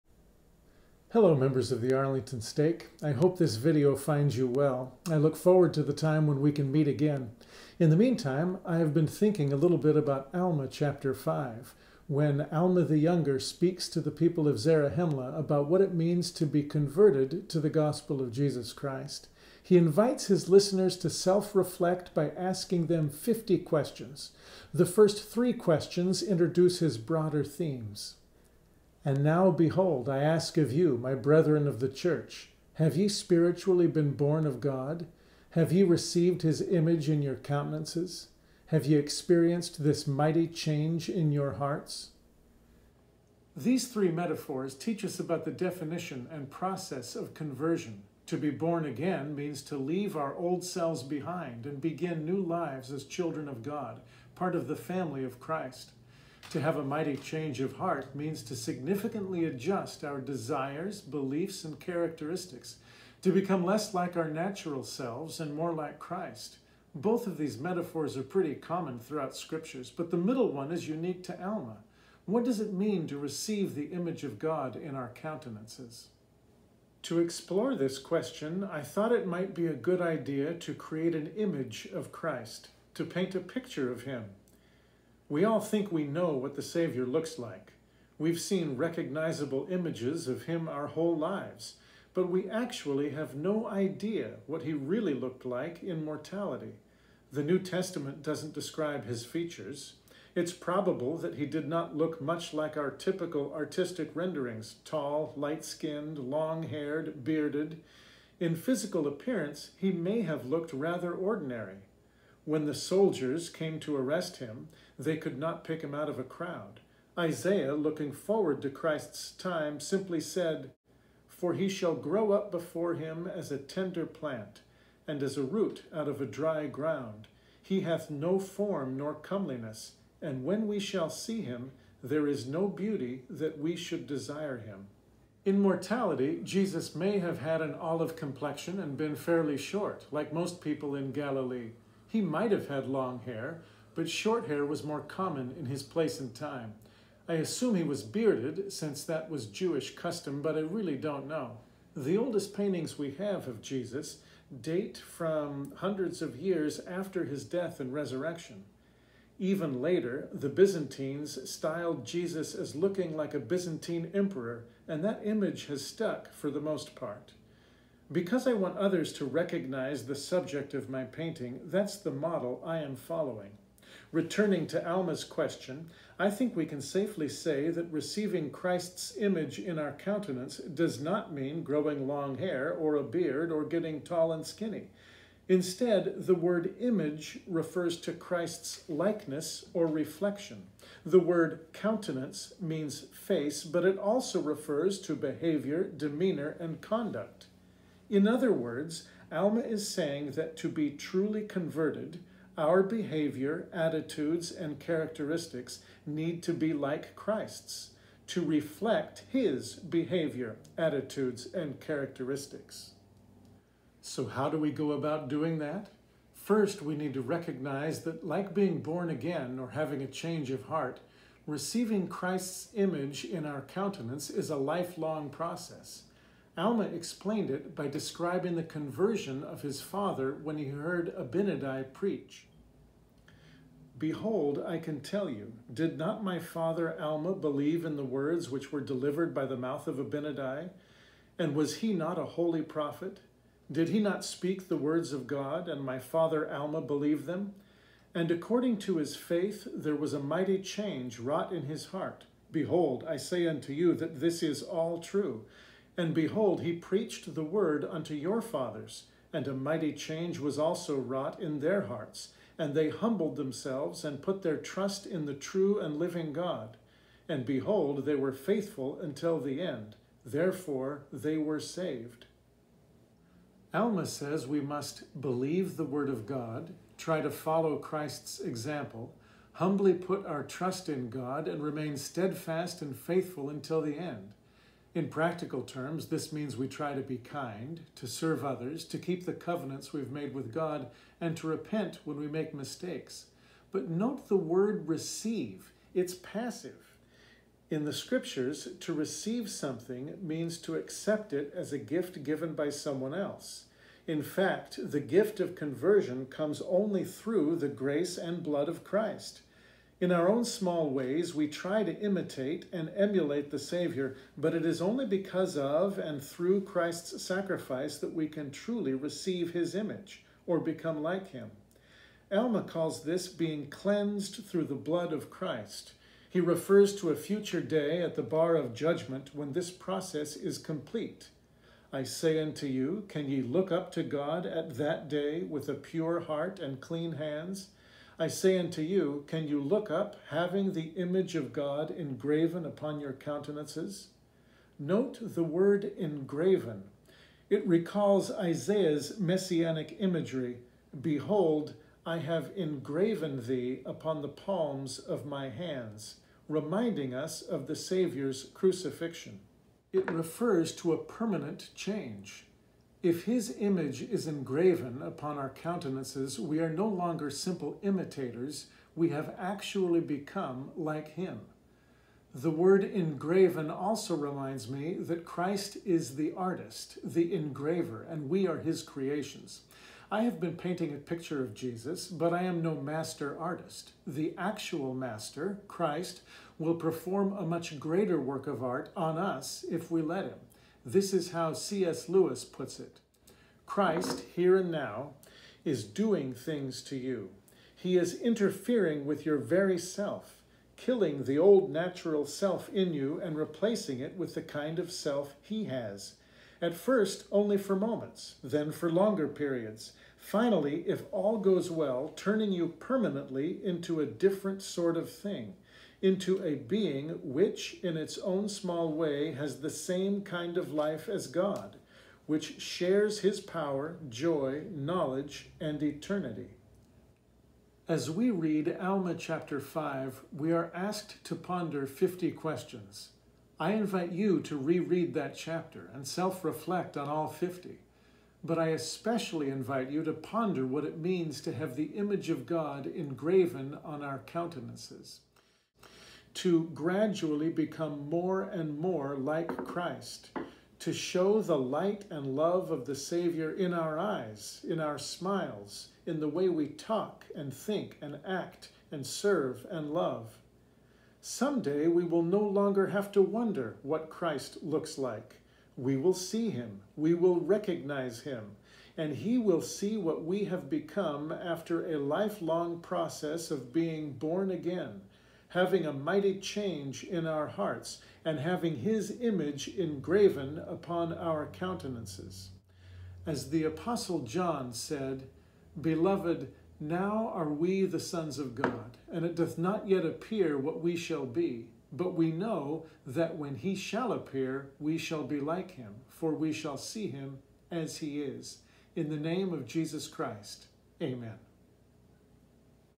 Image of Christ - A Message from President Williams
President Williams paints a picture of Jesus and explains what it means to have His image engraven upon our countenances.